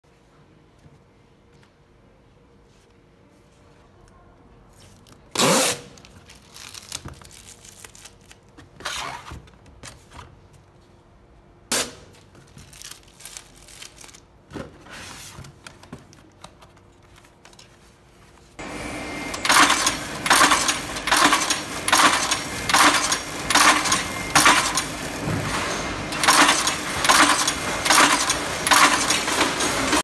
Custom size color white correx box -Huiyuan source manufacturer
Correx box is currently the most widely used box. Many overseas customers send us design drawings and let us be responsible for completing the design. Or simply customize to the size, color and thickness they need. It is also called correx box, corflute box, PP flute box, danpla bin and so on. Coroplast containers are made of PP hollow sheets with a lattice-like structure. PP hollow boxes can be used for packaging, warehousing and logistics turnover.